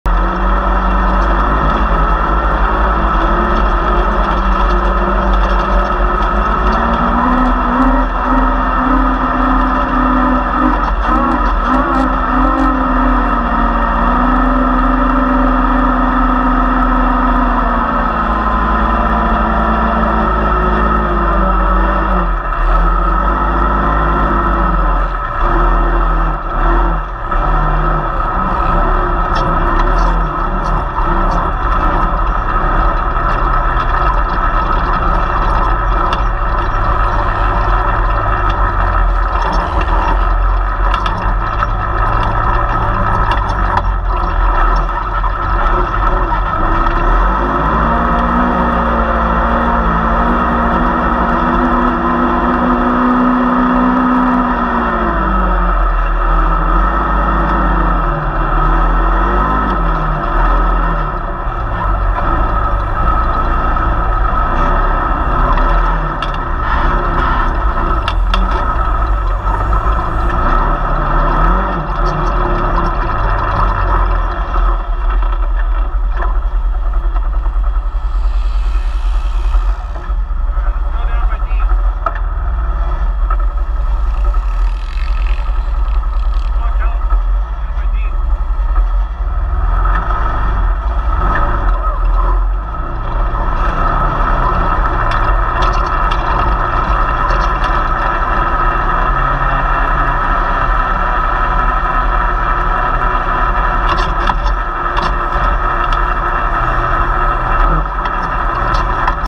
Blowsand2
Gavin's first time up Blowsand on 2 wheels and Kellen's first time making it up on a quad.